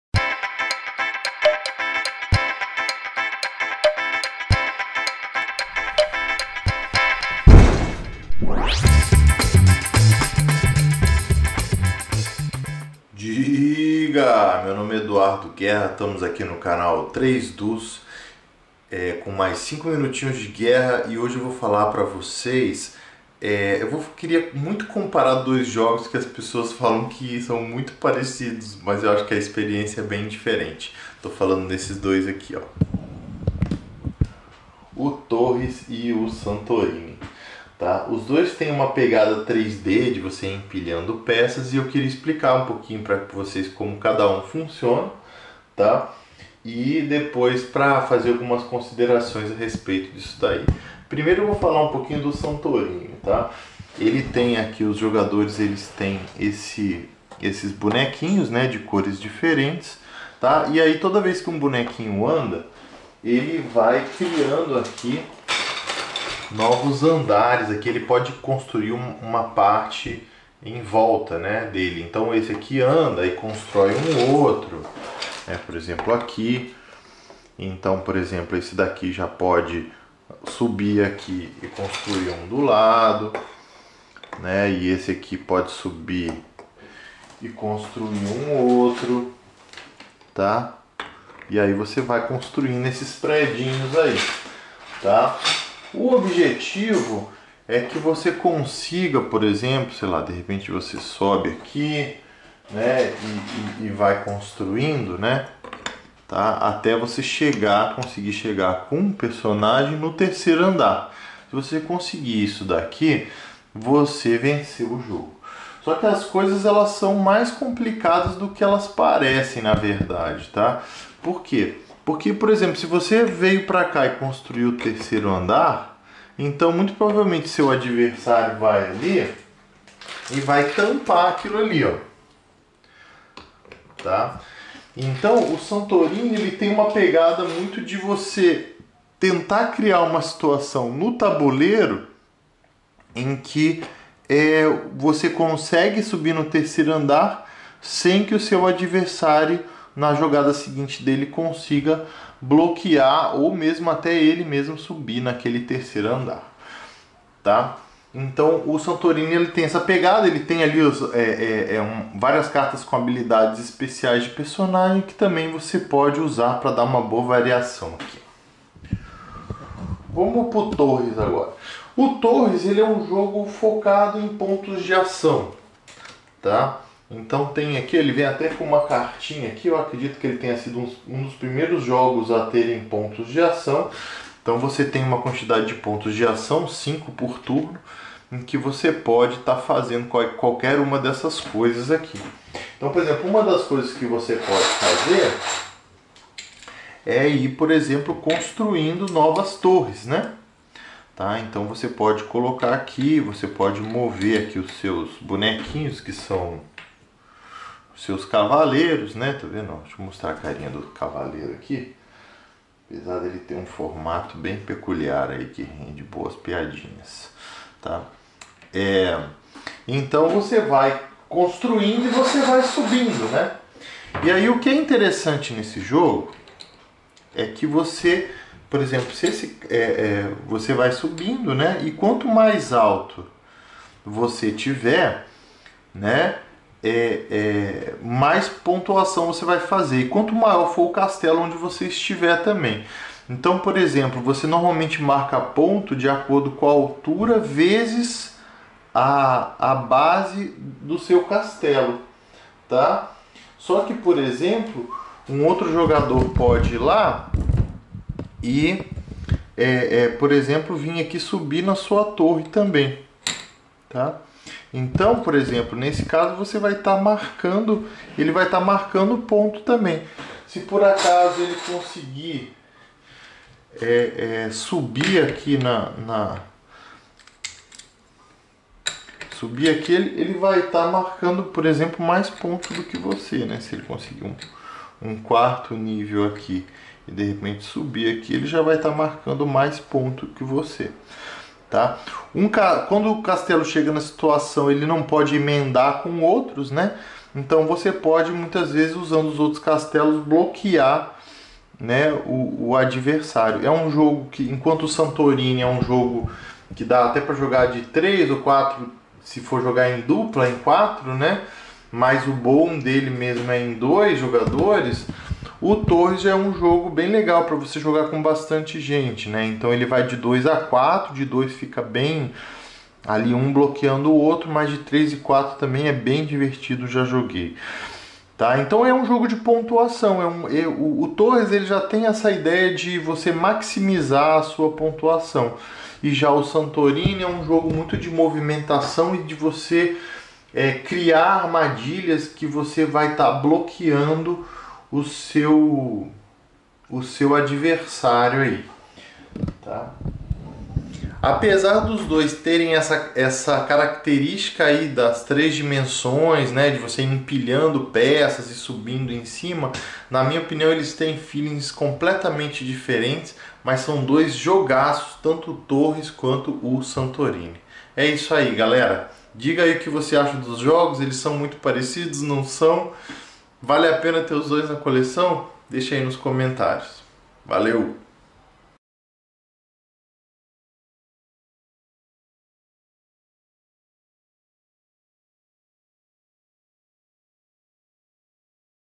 5 Minutos de Guerra - Ep. 28 - "Santorini x Torres"
Neste vídeo Edu Guerra fala desses dois jogos que tem temáticas parecidas. Da uma conferida.